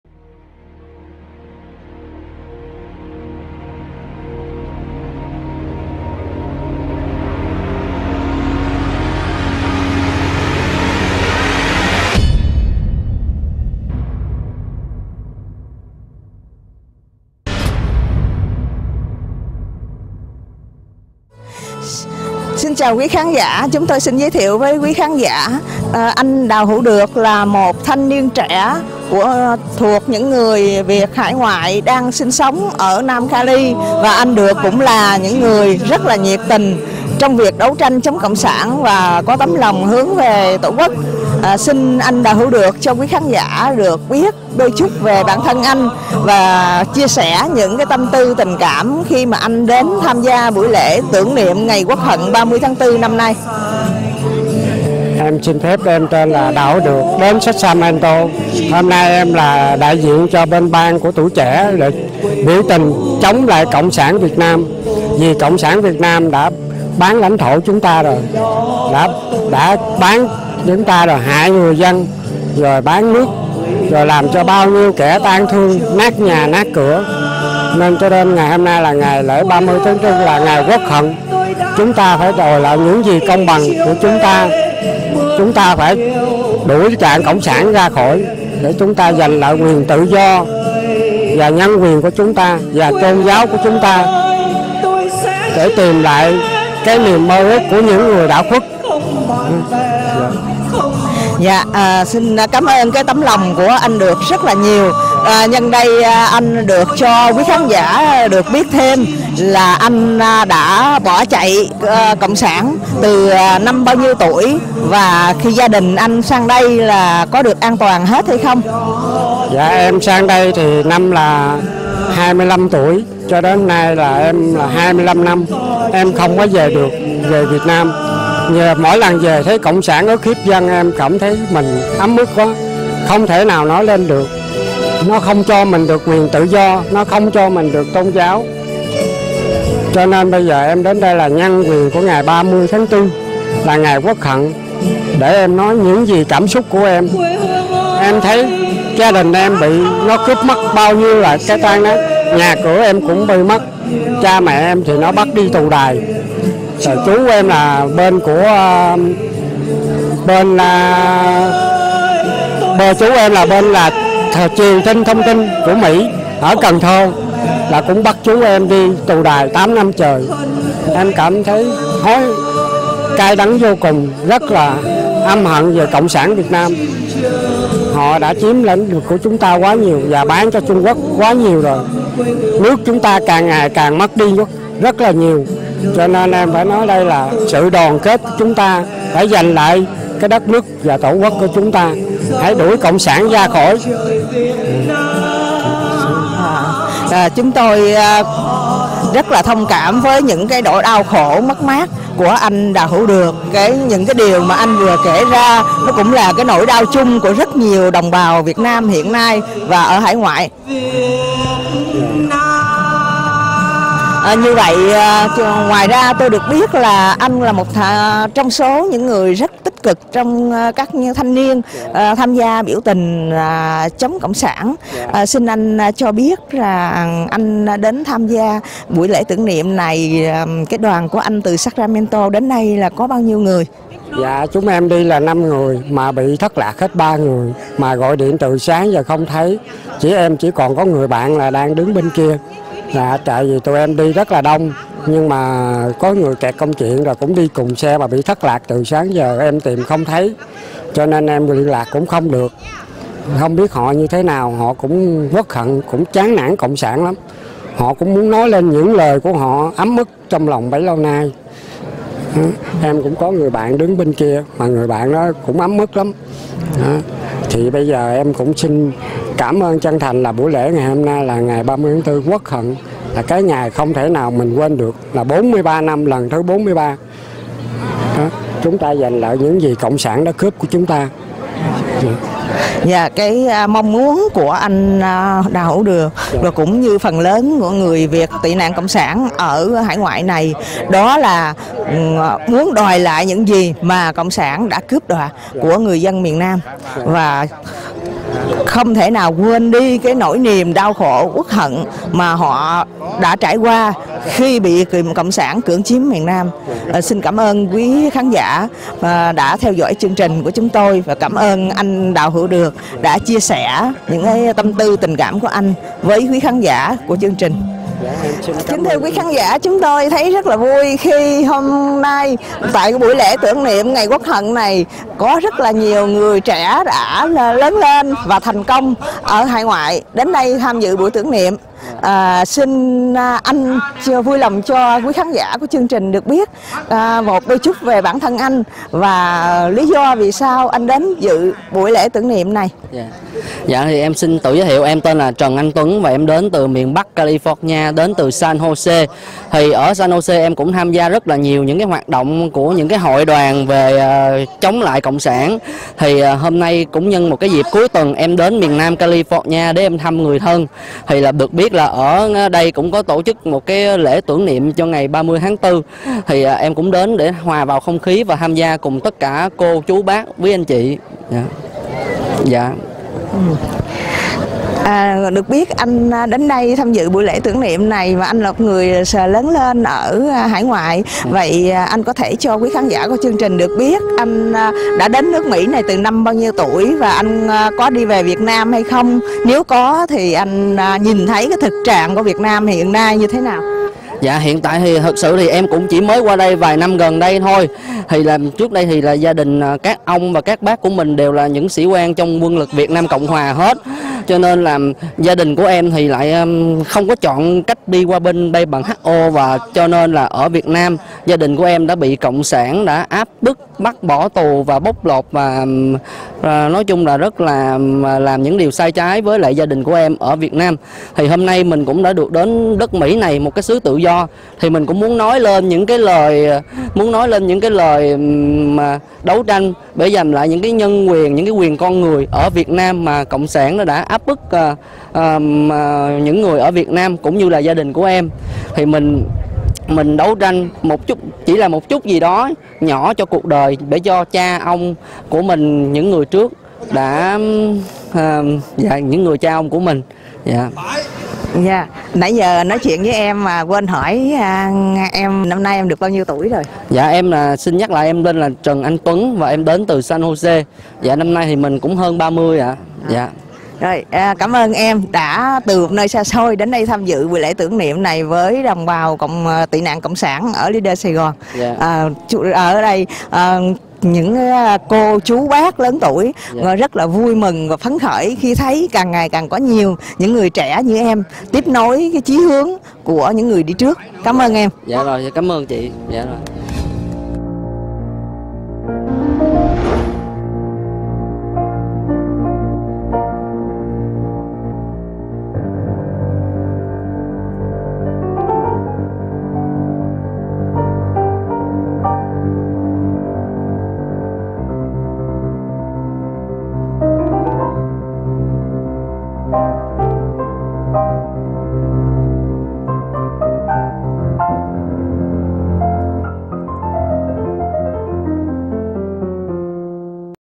Tạ Phong Tần Phỏng Vấn Giới Trẻ Dự Lễ Tưởng Niệm Ngày Quốc Hận 30/4 Tại TĐ Chiến Sĩ Việt Mỹ
Little Saigon (VanHoaNBLV) – Lễ Tưởng Niệm Ngày Quốc Hận 30 Tháng Tư năm nay 2018 do Cộng Đồng Người Việt Quốc Gia Nam California và Trung Tâm Điều Hợp Tây Nam Hoa Kỳ Tập Thể Chiến Sĩ VNCH cùng tổ chức đã diễn ra một cách trang nghiêm và long trọng với các vị đại diện dân cử, nhiều hội đoàn, đoàn thể đến dâng hương, đặt vòng hoa tưởng niệm cùng đông đảo đồng hương tham dự vào chiều tối ngày 29 tháng Tư Năm 2018 trong khuôn viên Tượng Đài Chiến Sĩ Việt Mỹ,  Số 14180 All American Way, Westminster, Miền Nam California, Hoa Kỳ.

Nhân dịp này Nhà báo Tạ Phong Tần, Cựu Tù Nhân Chính Trị, Nhà Báo Tự Do, Blog Sự Thật Và Công Lý đã phỏng vấn hai bạn thuộc giới trẻ là Đào Hữu Được, đến từ Sacramento và Họa sĩ Trần Anh Tuấn, đến từ San Jose về cảm nghĩ của các bạn khi tham dự lễ tưởng niệm ngày Quốc Hận 30 tháng Tư này.